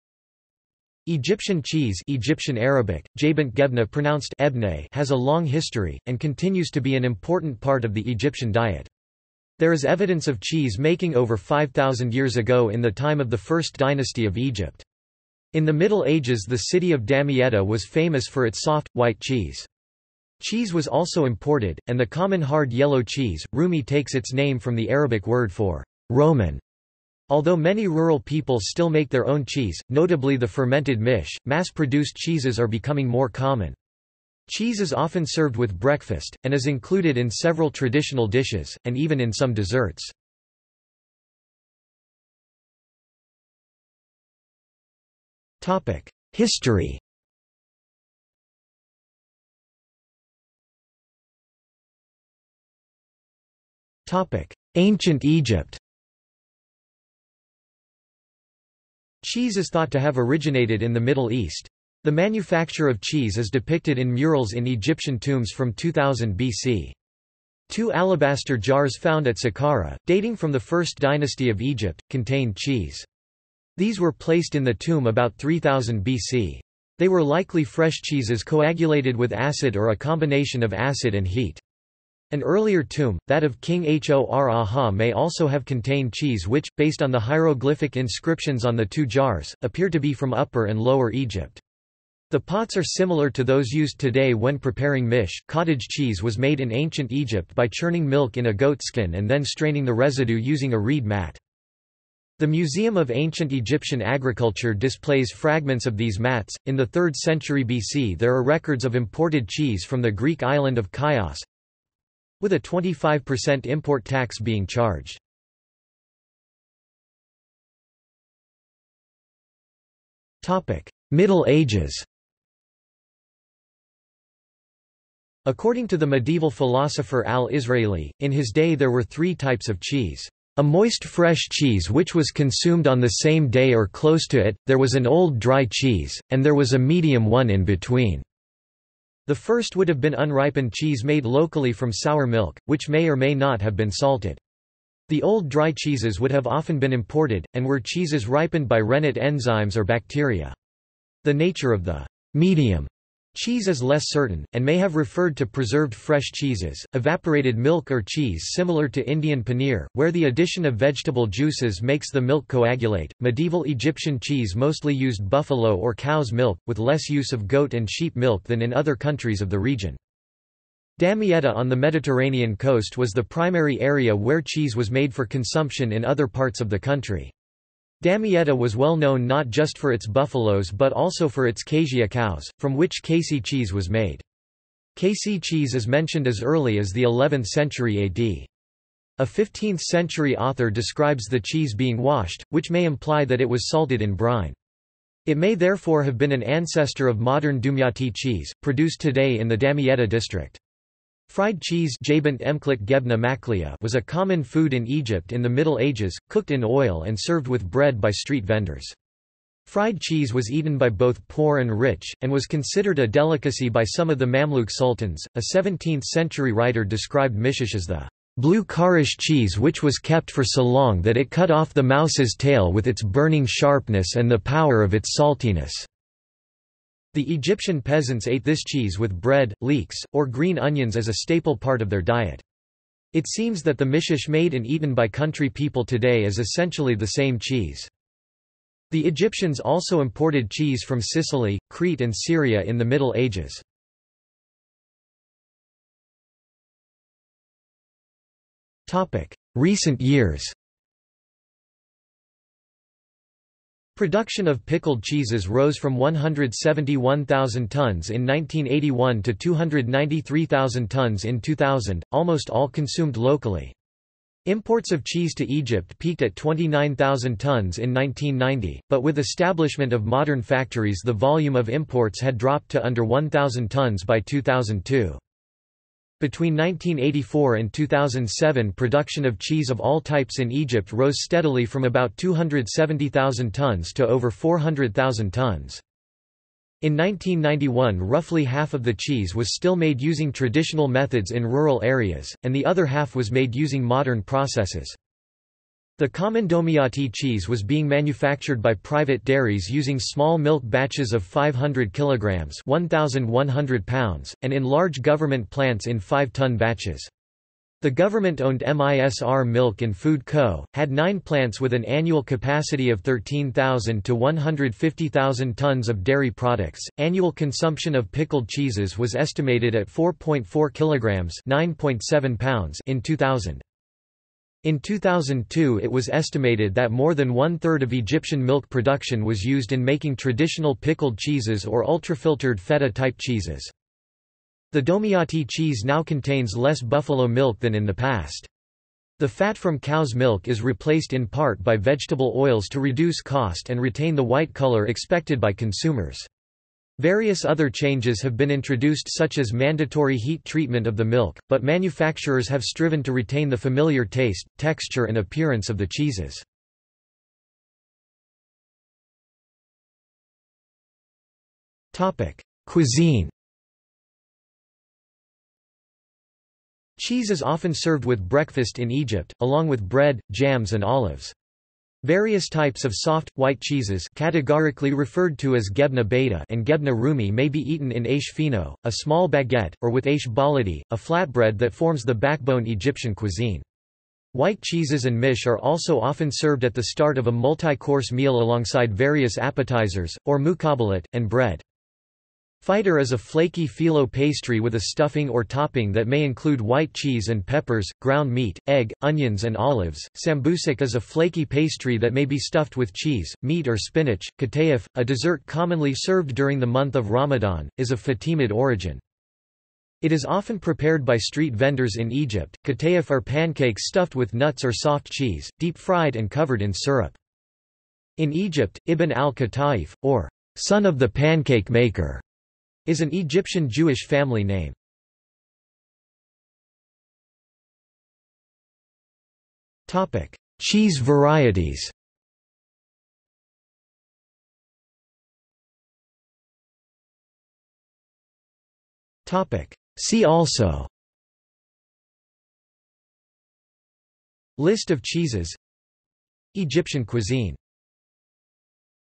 Egyptian cheese | Wikipedia audio article
This is an audio version of the Wikipedia Article:
Egyptian cheese

00:00:42 1 History
00:00:50 1.1 Ancient Egypt
00:01:57 1.2 Middle Ages
00:04:05 1.3 Recent years
00:05:58 2 Cuisine
00:07:06 3 Cheese varieties
00:07:15 4 See also

- Socrates

SUMMARY
Egyptian cheese (Egyptian Arabic: جبنة‎ gebna  pronounced [ˈɡebnæ]) has a long history, and continues to be an important part of the Egyptian diet. There is evidence of cheese-making over 5,000 years ago in the time of the First Dynasty of Egypt. In the Middle Ages the city of Damietta was famous for its soft, white cheese. Cheese was also imported, and the common hard yellow cheese, rumi takes its name from the Arabic word for "Roman". Although many rural people still make their own cheese, notably the fermented mish, mass-produced cheeses are becoming more common. Cheese is often served with breakfast, and is included in several traditional dishes, and even in some desserts.